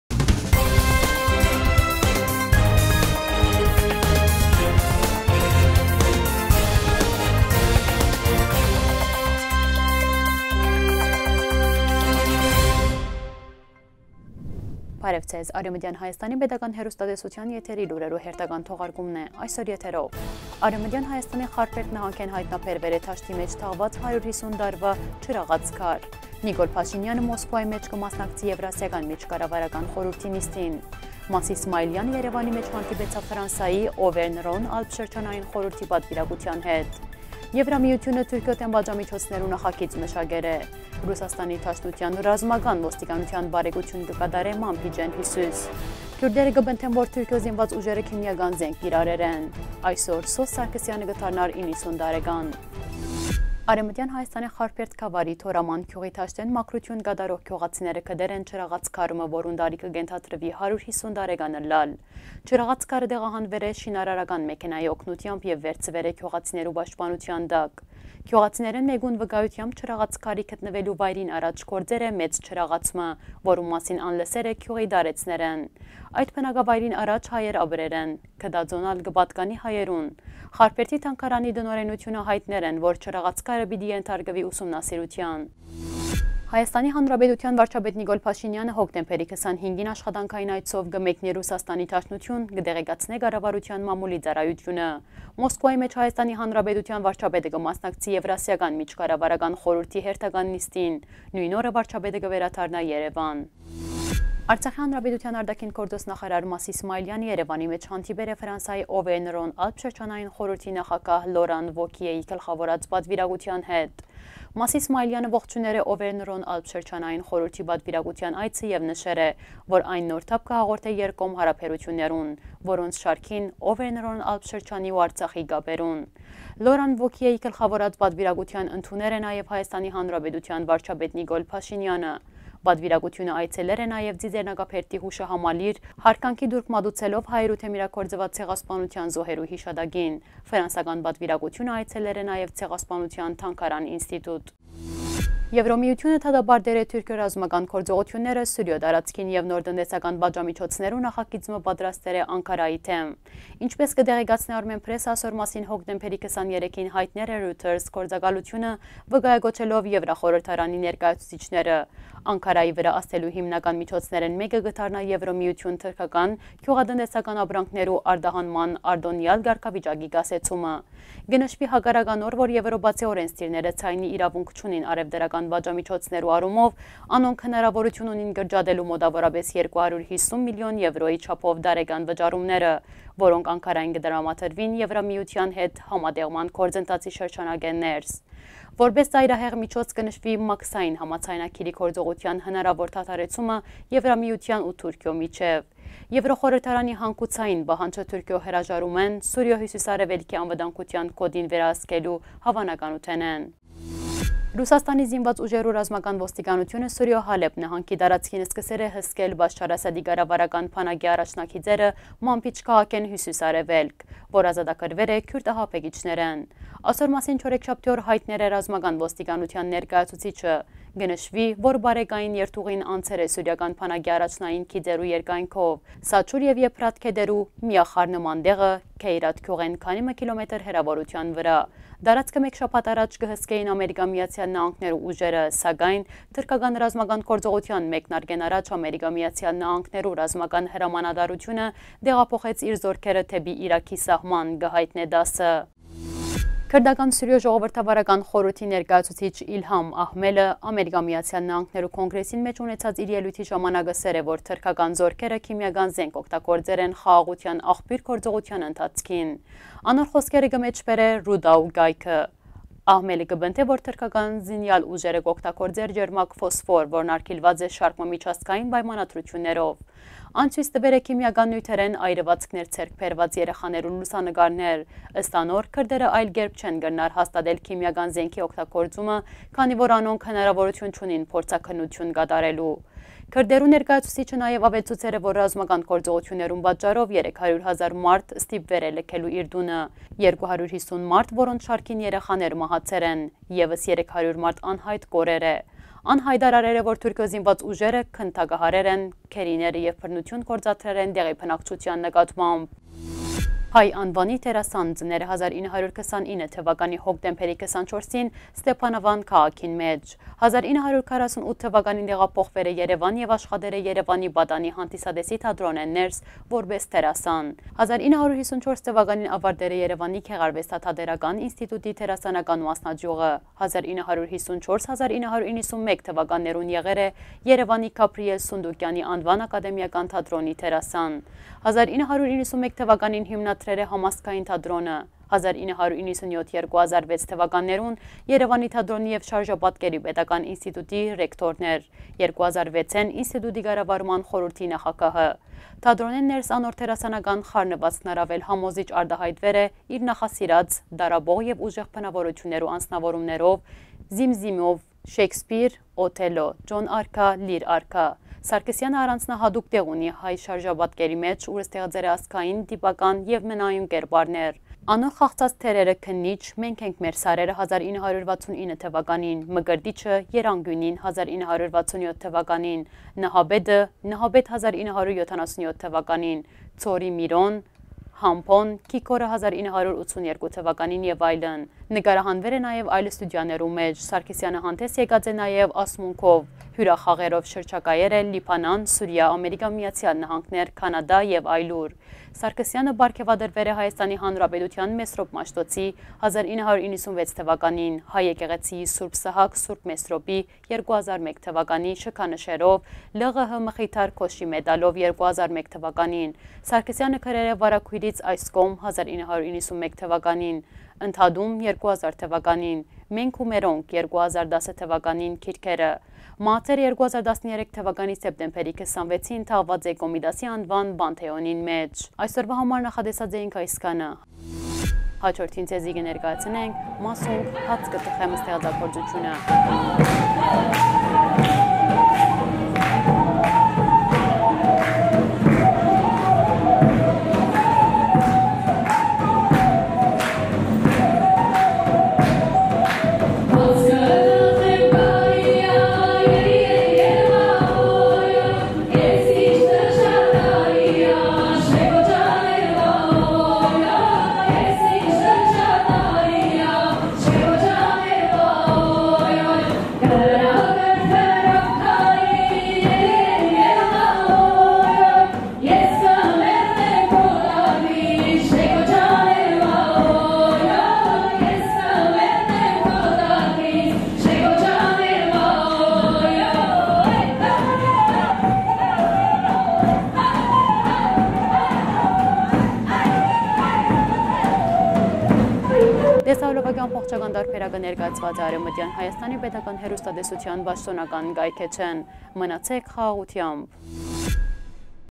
ԵՄ-ն Թիւրքիոյ դէմ պատժամիջոցներու նախագիծ մշակեր է:Լրահոս 2019-10-24
1.Արեւմտեան Հայաստանի Խարբերդ նահանգէն յայտնաբերուեր է դաշտի մէջ թաղուած 150 տարուայ ջրաղացքար
2.Նիկոլ Փաշինեանը Մոսկուայի մէջ կը մասնակցի Եւրասիական միջկառավարական խորհուրդի նիստին
3.Մասիս Մայիլեանը Երեւանի մէջ հանդիպեցաւ Ֆրանսայի Օվերն-Ռոն-Ալպ շրջանային խորհուրդի պատուիրակութեան հետ
4.ԵՄ-ն Թիւրքիոյ դէմ պատժամիջոցներու նախագիծ մշակեր է
5.ՌԴ ՌՈ պարեկութիւն կը կատարէ Մանբիջէն հիւսիս
6.Քիւրդերը կը պնդեն, որ Թիւրքիոյ զինուած ուժերը քիմիական զէնք կիրառեր են
7.Այսօր Սօս Սարգսեանը կը դառնար 90 տարեկան